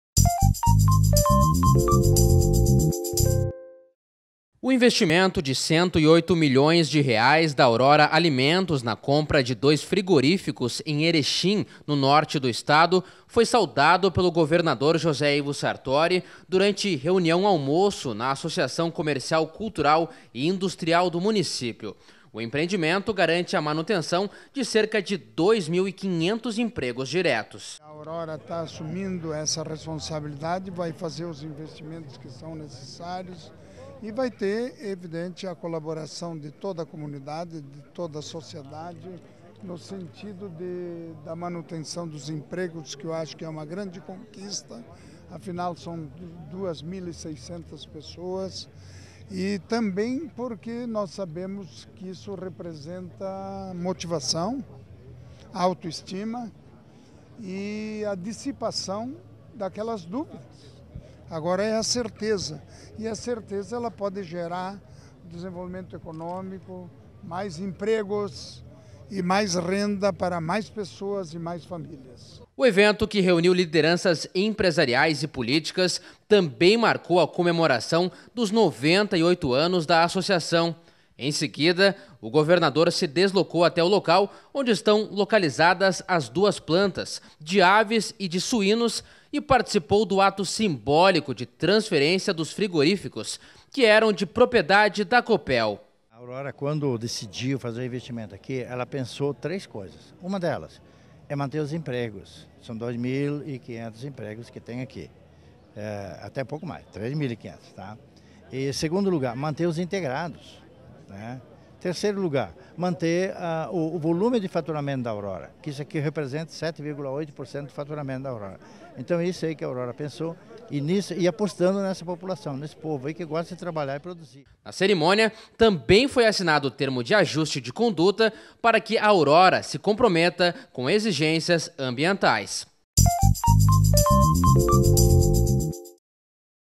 Sartori reforça incentivo a empreendedores ao inaugurar planta da Aurora Alimentos
Ao celebrar a instalação definitiva da Aurora Alimentos em Erechim, nesta sexta-feira (24), o governador José Ivo Sartori reforçou que o incentivo aos empreendedores é uma prioridade no Rio Grande do Sul. A empresa de Chapecó (SC), terceiro grupo agroindustrial de carnes do país, assumiu definitivamente no início do mês duas unidades frigoríficas (de aves e suínos) da Cooperativa Tritícola Erechim (Cotrel), em investimento de R$ 108 milhões.